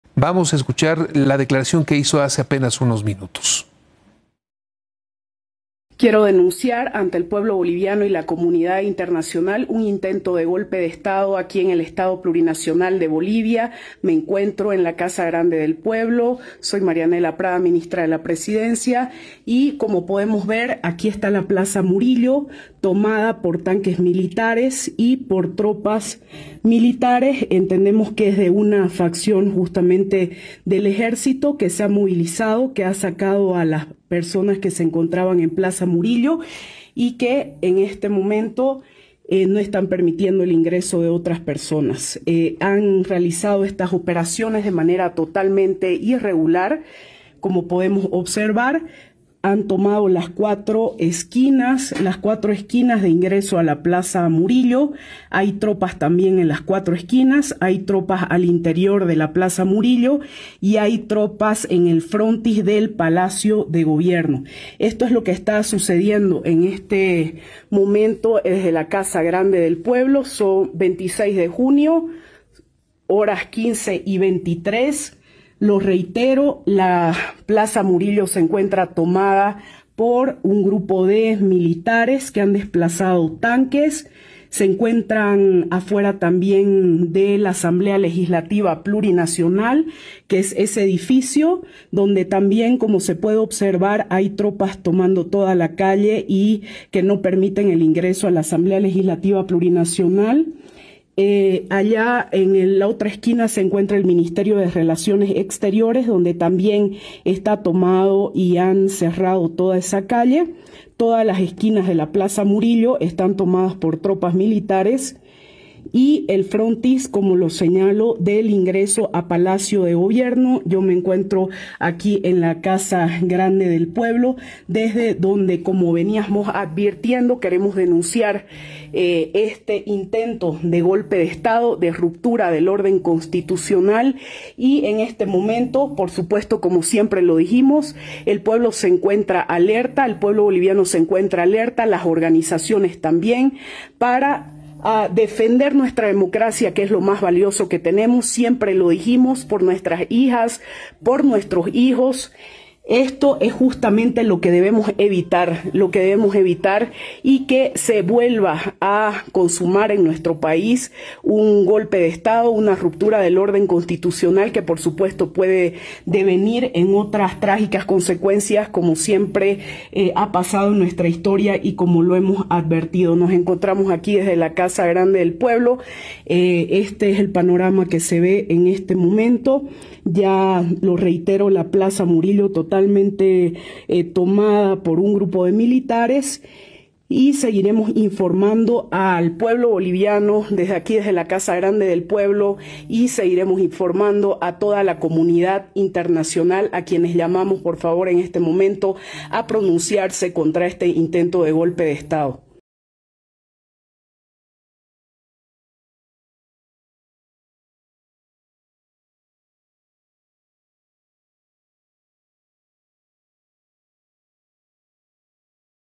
María Nela Prada denuncia toma de la Plaza Murillo, en Bolivia - Noticias MX
La ministra de la presidencia de Bolivia, María Nela Prada, denunció un intento de golpe de estado en el país sudamericano. Denunció el despliegue de fuerzas militares en la Plaza Murillo y otros puntos de la capital boliviana.